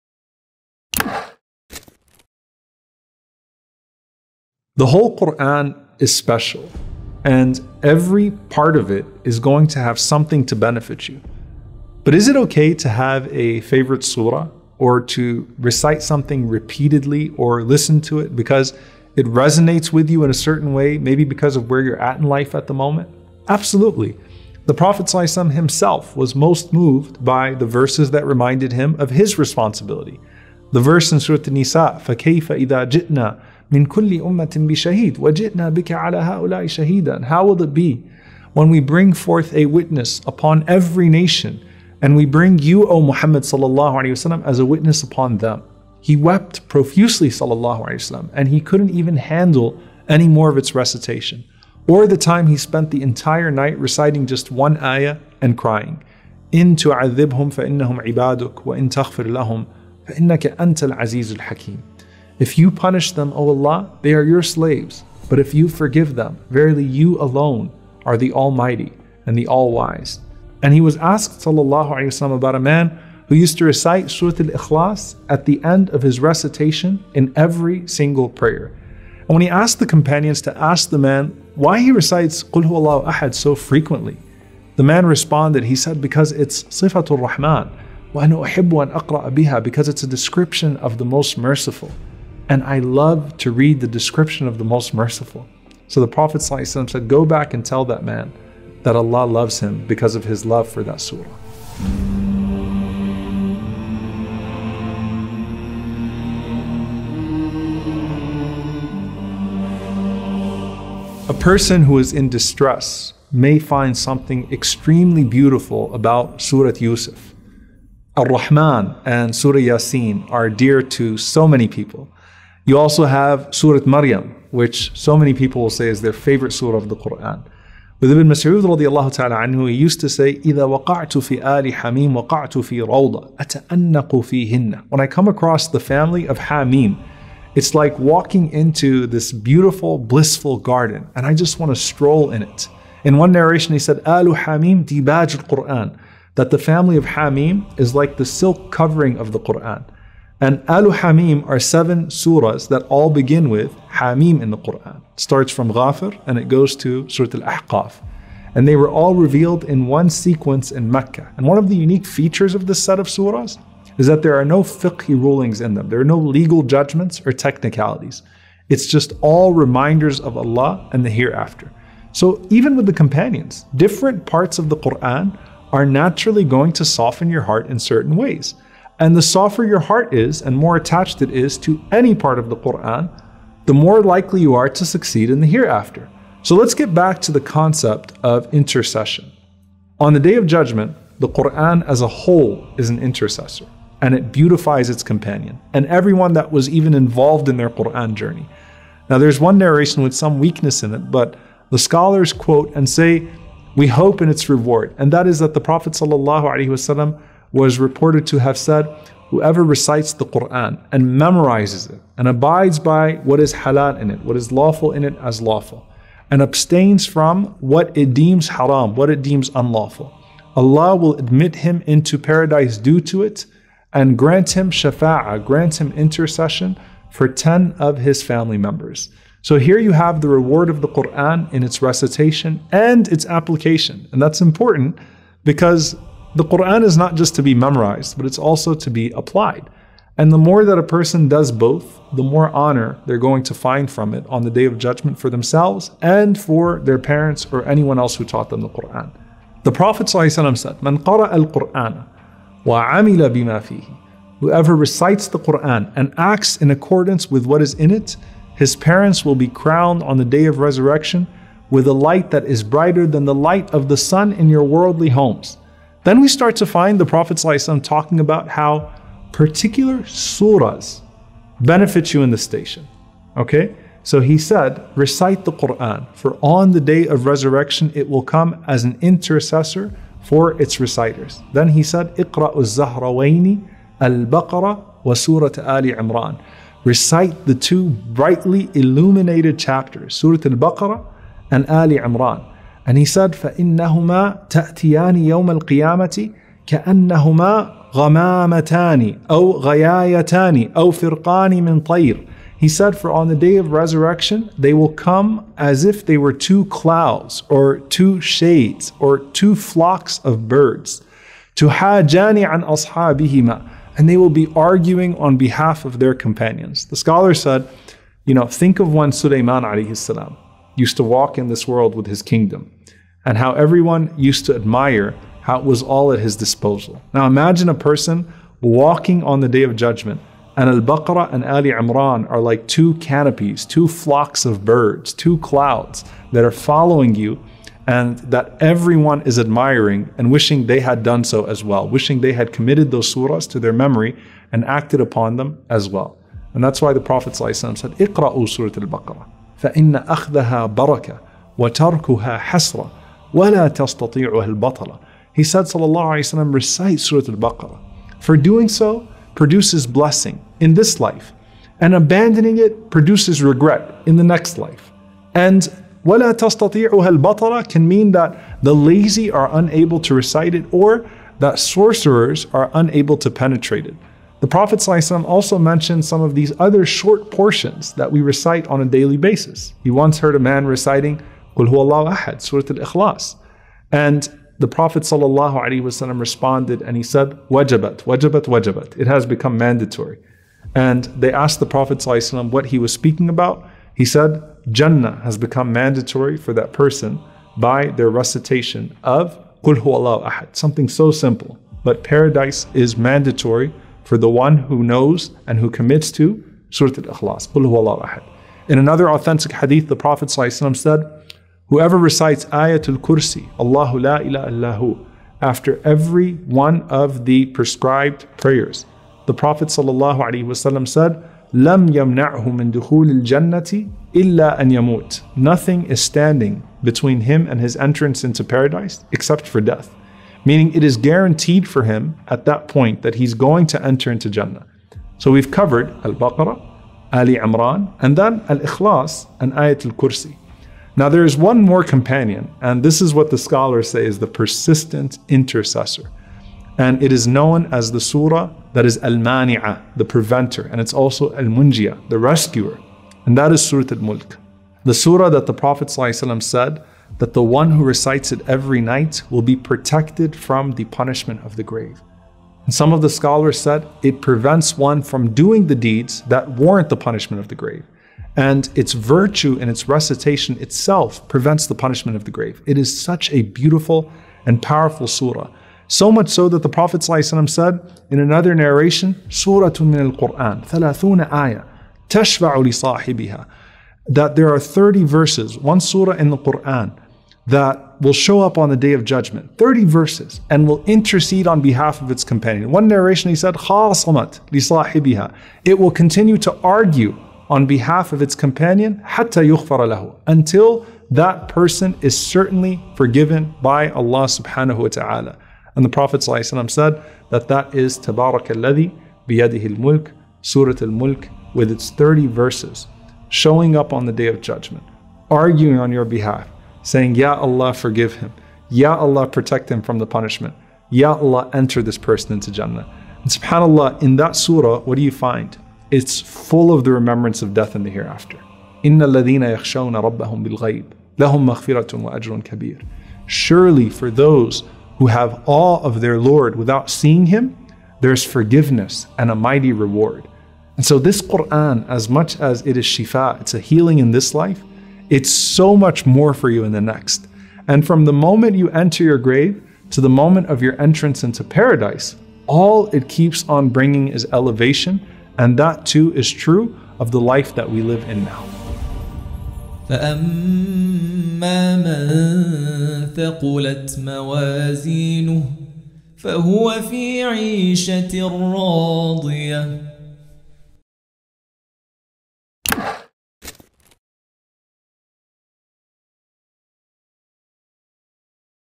Sizi Kurtaracak Sureler | Yolumuzu Aydınlatan Ameller 17. Bölüm | Omar Suleiman
Özel ve kendisine özgü, şifa kaynağı Kuran’ın her bölümü dünya ve ahiret için bir fayda sağlayıcıdır. Peki, sizce favori bir sureye sahip olmak, bir sureyi ya da bir yeri tekrar tekrar okumak doğru mudur? Hesap Günü serisinin 17.bölümünde bu ve bunun gibi sorulara yanıt bulabilirsiniz.

Altyazı Çevirmeni: Cahit Eren Dalsuna

Dünyanın her köşesinden Müslümanlarla ilgili olumlu, güzel ve ilham verici haberleri öğrenmek için